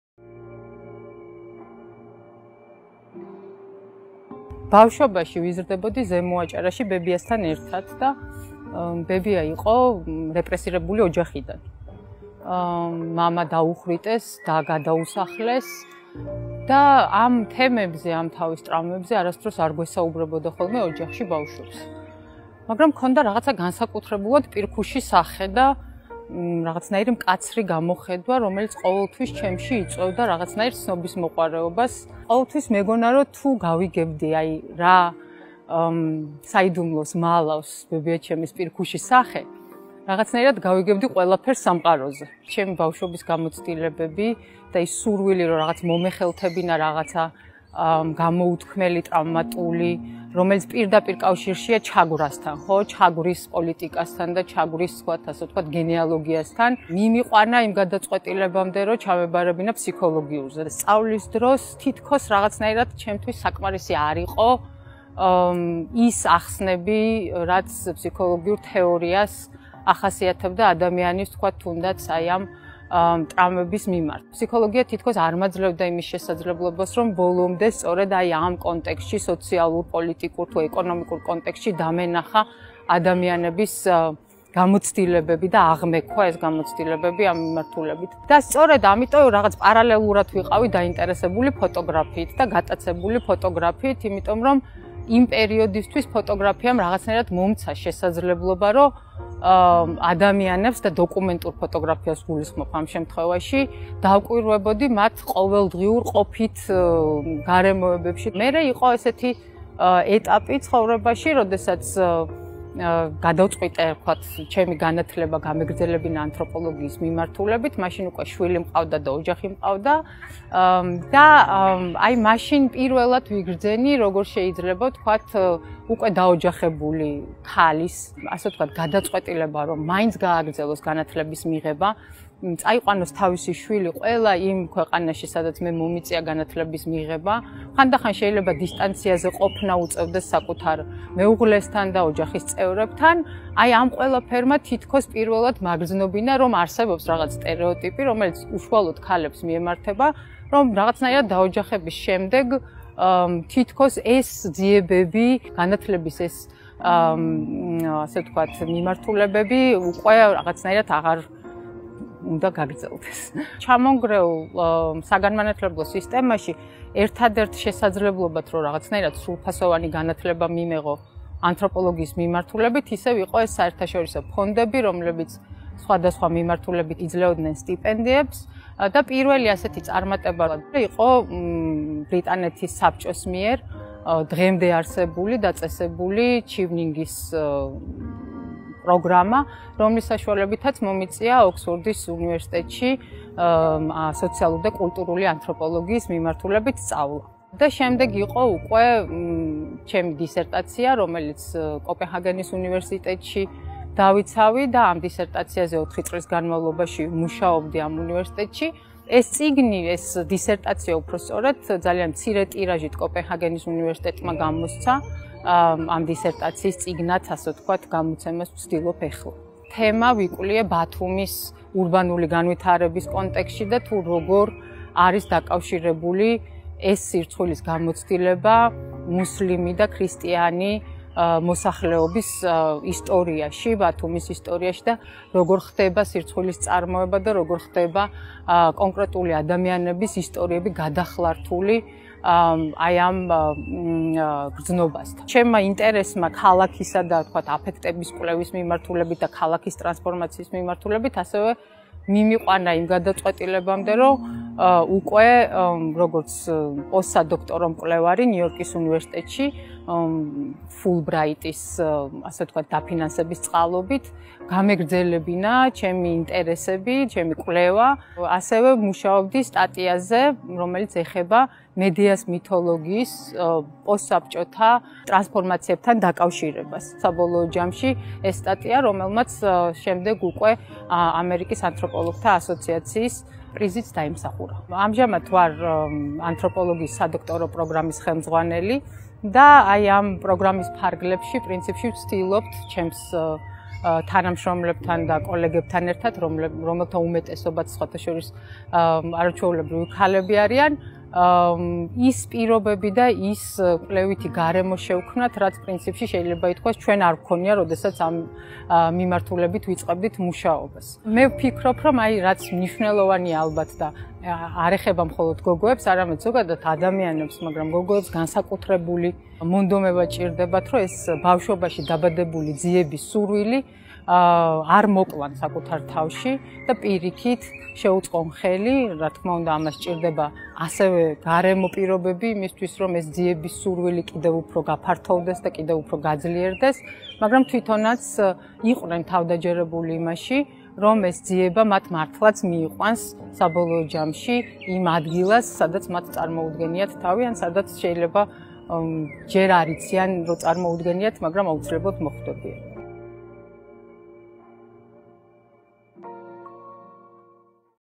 ქალები მეცნიერებაში - თამთა ხალვაში
💬 „ზოგადად ადამიანებს და გოგოებს განსაკუთრებით, დიდი მონდომება სჭირდებათ, რომ ბავშვობაში გაჩენილი ძიების სურვილი არ მოკლან საკუთარ თავში და პირიქით, შესაბამის გარემო-პირობებთან ერთად, ხელი შეუწყონ საკუთარ თავს, რათა ეს ძიების სურვილი კიდევ უფრო გაფართოვდეს და გაძლიერდეს და იყვნენ თავდაჯერებულები, რომ ეს ძიება მართლაც მიიყვანთ იმ ადგილას, სადაც საკუთარი თავი  წარმოუდგენიათ“.

🔴 თამთა ხალვაში ილიას სახელმწიფო უნივერსიტეტის პროფესორი ანთროპოლოგიის მიმართულებით, ანთროპოლოგიის სადოქტორო პროგრამის ხელმძღვანელი, საკუთარი სამეცნიერო კარიერული ზრდის ისტორიას გვიზიარებს.